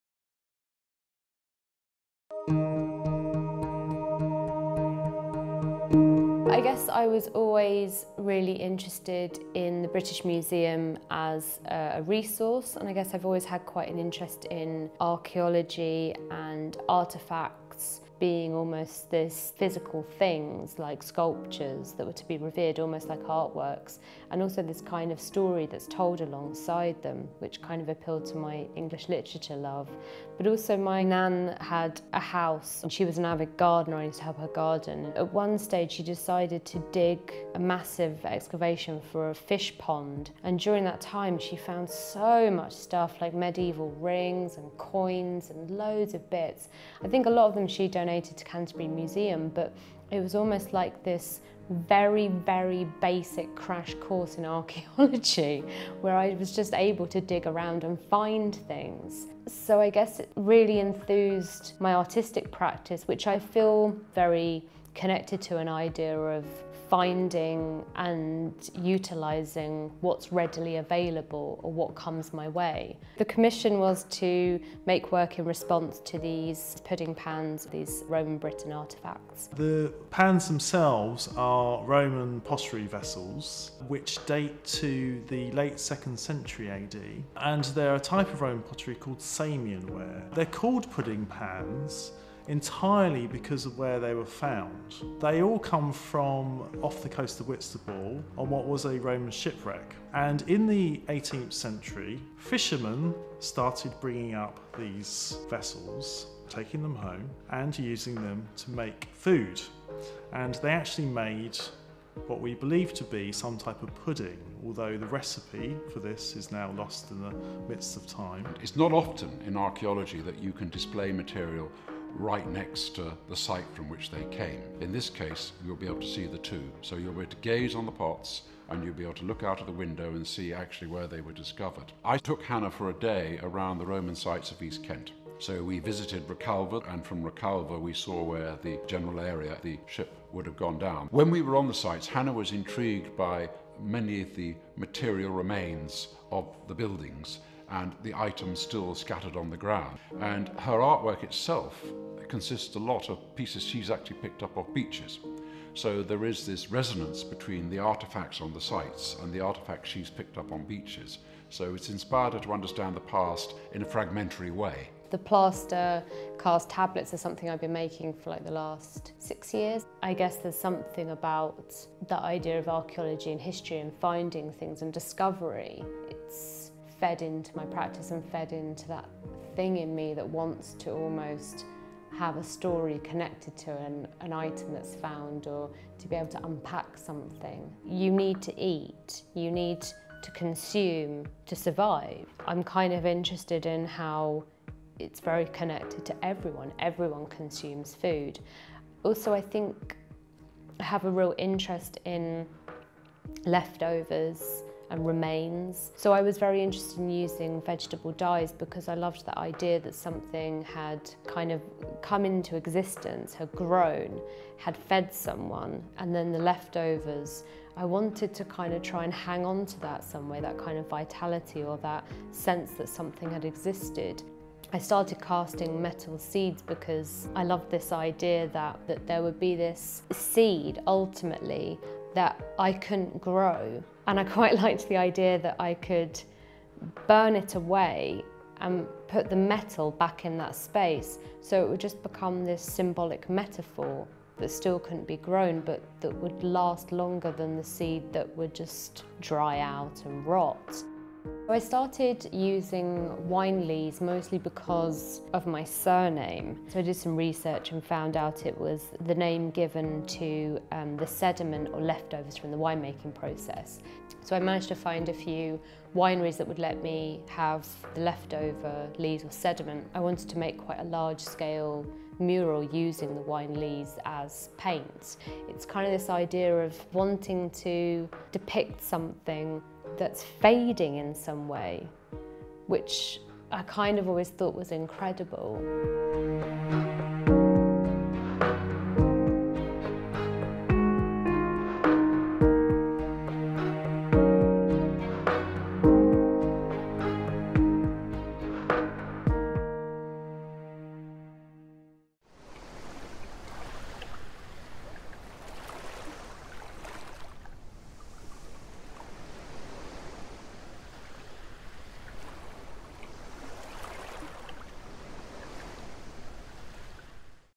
Hannah Lees "The Passage of Time" Art Installation | Turner Contemporary
In this video artist Hannah Lees talks about her exhibition The Passage of Time, and art journey, surrounding her love of archeology, and how it influences her work. Hannah paints using Wine Lees to represent a passing of time, decay, and maturity.

Her journey took her through centuries of archeology, and through Thanet and Margate's history itself. From Roman artefacts, right through the modern-day.

The Passage of Time was shown at Turner Contemporary in 2017, and tells a story of how we all connect ourselves to the earth itself.

About the Artist:
Hannah Lees’ is a female artist whose work encompasses painting, drawing, sculpture, installation, textiles, ceramics, internet art, performance, writing, sound and video.

Lees investigates ideas of cycles: constancy and mortality; the sense that things come to an end and the potential for new beginnings.

This constancy, be it in religion, science, history or in organic matter, is visible in her practice through her attempts to make sense of and recognise traces of life.

Through appreciating this, her work is focused towards an understanding of the essential nature of the materials she uses.

She completed a Postgraduate Diploma at Chelsea College of Arts and a Foundry Residency at the Royal College of Art. She was awarded the Micro Residency at the Edinburgh Sculpture Workshop (Scotland).

Previous exhibitions include:

Vital Materiality at Assembly Point, Companion Planting at The Living Room (both London), Overview at SAMUEL Gallery (Chicago, United States of America) and the Turner Contemporary and the British Museum Commission (Margate, United Kingdom).

BA Fine Art Painting

Keep up to date with the latest news from Turner Contemporary?